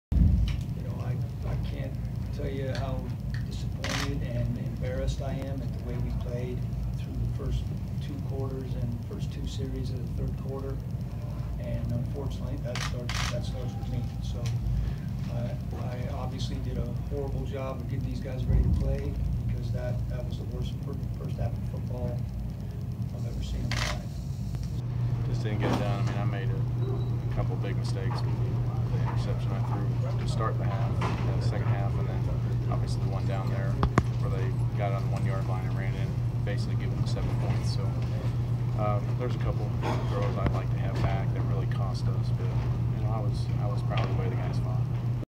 Bucs' Koetter: "Disappointed And Embarrassed" By First Half
Tampa Bay Buccaneers left disappointed after rallying from a 31-point hole and falling short in a 38-33 loss to Arizona Cardinals. (Oct. 15, 2017).